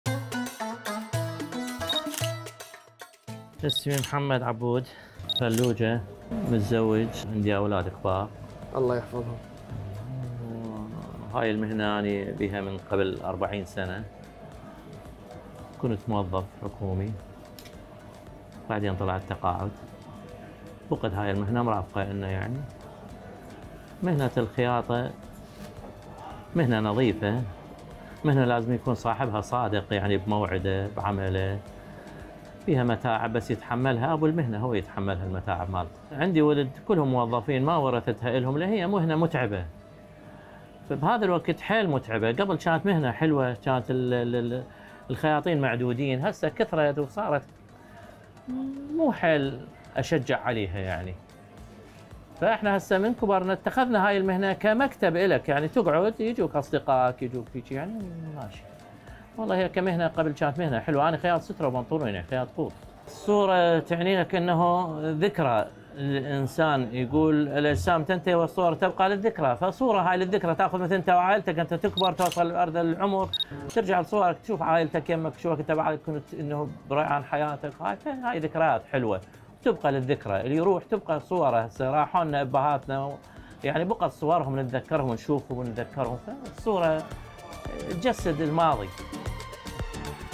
"الصورة تجسد الماضي" خياط الملابس يتحدث عن أهمية الصور له وللإنسان مع مصور الشارع محمد العاني
في مدينة الفلوجة بمحافظة الأنبار العراقية، يترجل محمد العاني مصور الشارع في أنحاء المدينة، ليلتقط صوراً من الشوارع، ويوثق الحياة اليومية للبسطاء في الفلوجة، يمر على تاجر الدهن الذي تعلم المهنة من الأمهات، ومنه إلى تاجر التمور، وبائع الشاي، وحتى الخياط والنجار ومصلح الأدوات المنزلية، ويسجل مصور الشارع بعدسته كل ما تقع عليه عينه، حتى يوثق مدينة الفلوجة التي ولد فيها، ويترك صور الآن؛ للشباب الغد ليروا كيف كان أهلها يعيشون على بساطتهم، ودون تكلف؛ في رحلة ثرية بالحكايات والصور.